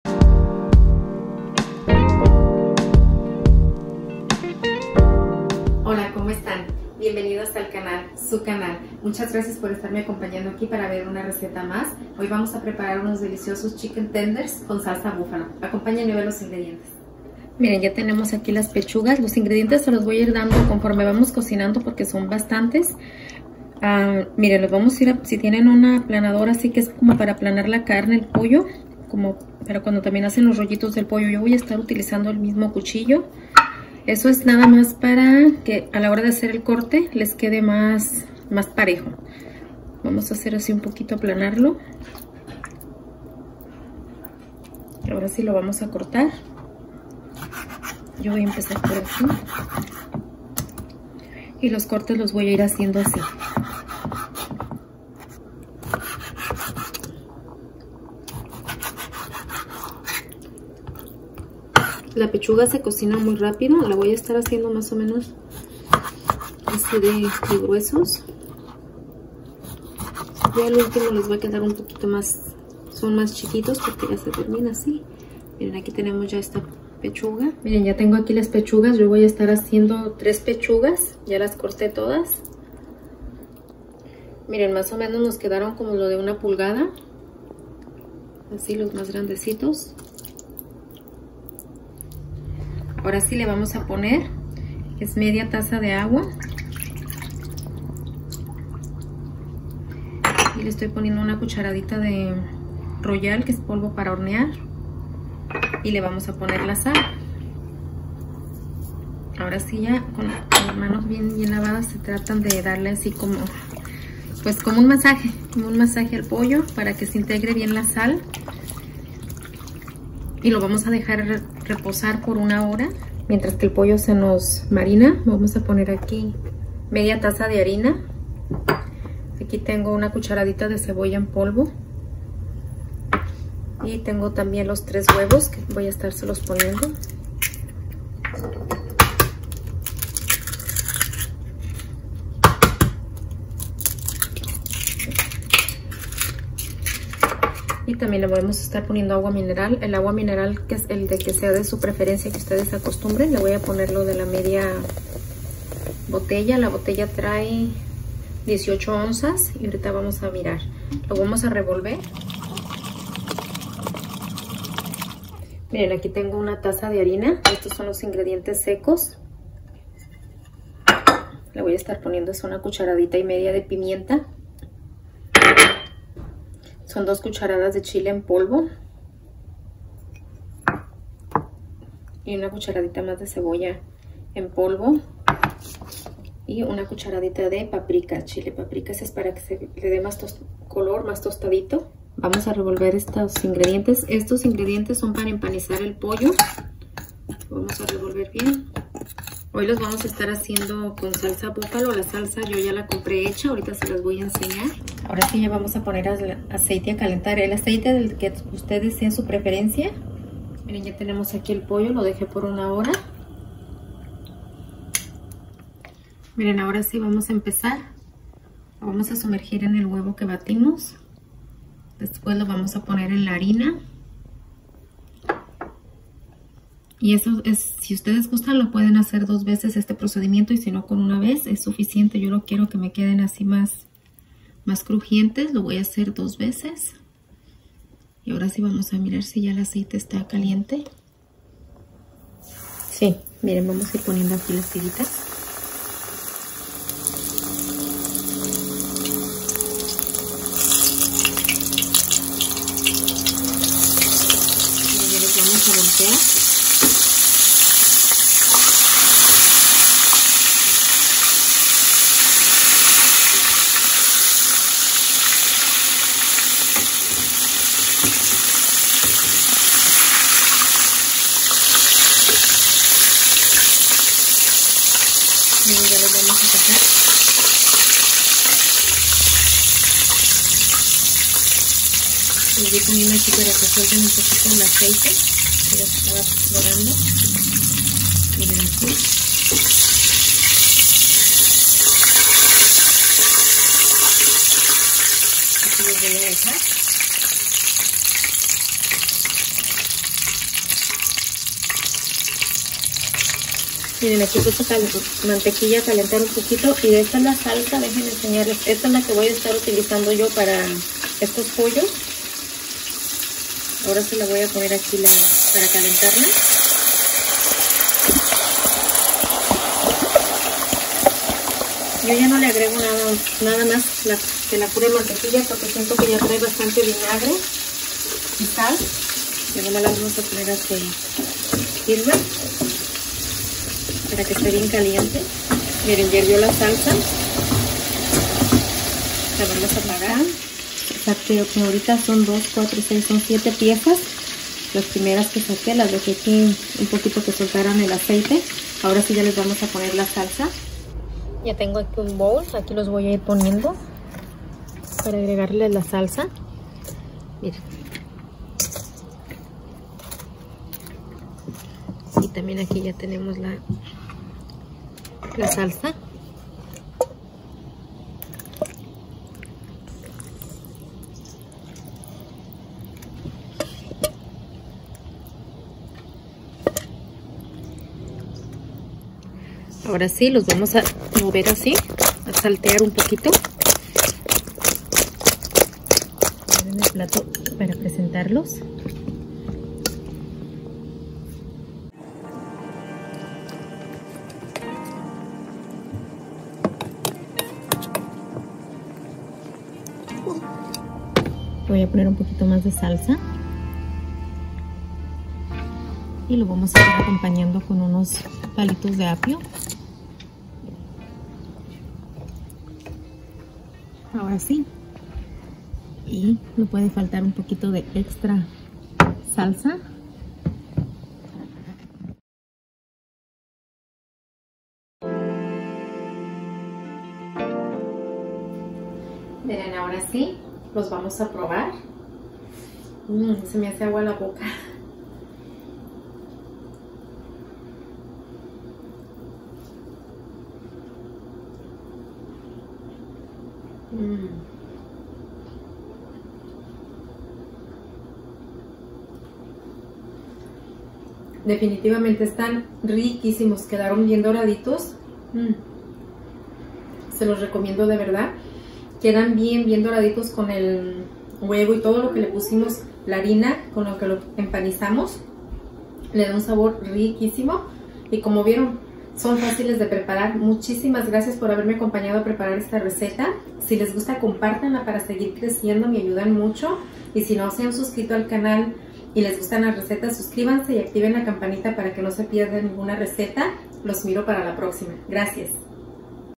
Chicken Tenders en Salsa Buffalo | Receta Paso a Paso
Hola a todos! Espero se encuentren muy bien! Hoy vamos a preparar estos chicken tenders en salsa buffalo!

Ingredientes:

Paso #1:
-1/2 taza de agua
-2lb de pechuga de pollo
-1 cdta de sal
-1 cdta de royal
**sazonar por una hora**

Paso #2:
-2 huevos
-1/2 taza de harina
-1/2 lata de agua mineral
-1/2 cdta de sal
-1/2 cdta de cebolla en polvo

Paso#3:
-1 y media taza de harina
-1cdta de cebolla en polvo
-1 cdta de paprika
-1 cda de chile en polvo
-1 cdta de pimienta

Paso #4:
-2 tazas de salsa buffalo
-3 cdas de mantequilla sin sal

Ya viste mis ultimos videos? Te invito!

POLLO RELLENO CON VERDURAS AL HORNO | PASO A PASO (para los que no les gusta el pavo)